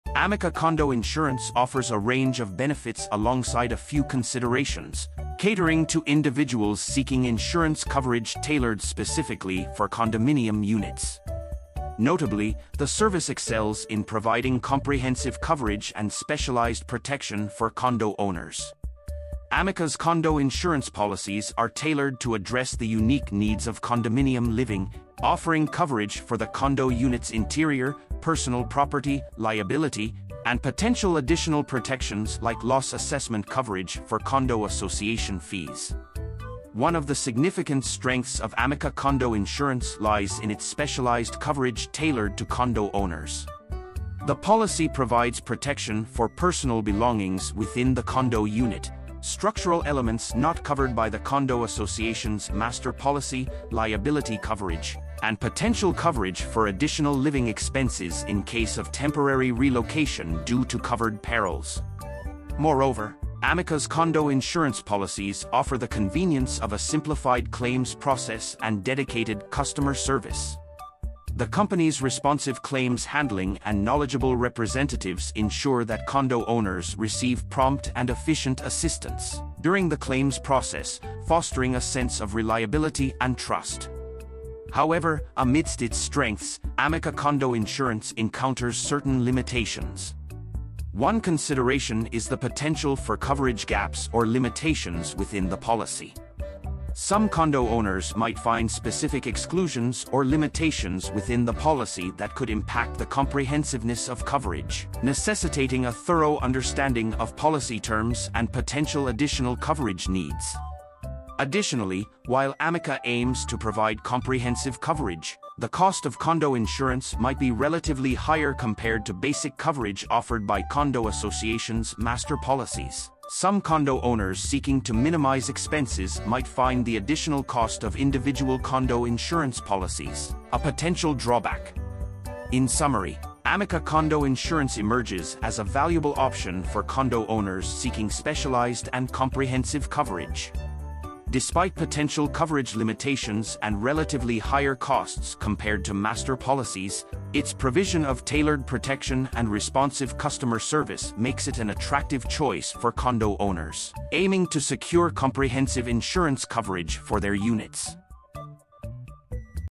🔥 Amica Condo Insurance Review: Pros and Cons
Amica Condo Insurance presents a range of advantages alongside a few considerations, designed to meet the specific insurance needs of condominium owners. Notably, the service excels in providing tailored coverage and specialized protection for condo units. Amica's condo insurance policies are crafted to address the unique requirements of condo living, encompassing coverage for the interior structure, personal belongings, liability, and potential additional protections such as loss assessment coverage for condo association fees.

One of the primary strengths of Amica Condo Insurance lies in its specialized coverage tailored explicitly for condo owners. The policy ensures coverage for personal possessions within the condo unit, structural elements not included in the condo association's master policy, liability protection, and potential coverage for additional living expenses during temporary relocation due to covered perils.

Moreover, Amica's condo insurance policies are applauded for their streamlined claims process and dedicated customer support. The company's efficient claims handling and knowledgeable representatives strive to provide condo owners with prompt and helpful assistance during the claims process, enhancing confidence and trust in the service.

However, amidst its strengths, Amica Condo Insurance faces certain limitations. One consideration is the potential for coverage limitations or exclusions within the policy. Some condo owners might discover specific exceptions or limitations within the policy that could affect the overall comprehensiveness of coverage, necessitating a careful review of policy terms and potential additional coverage needs.

Additionally, while aiming for comprehensive coverage, the cost of condo insurance might be relatively higher compared to basic coverage provided by condo associations' master policies. This additional expense might deter some condo owners looking to minimize costs and rely solely on master policies.

In summary, Amica Condo Insurance emerges as a valuable option for condo owners seeking tailored and comprehensive coverage. Despite potential limitations in coverage and relatively higher costs compared to master policies, its provision of specialized protection and responsive customer service makes it a suitable choice for condo owners aiming to secure comprehensive insurance coverage for their units.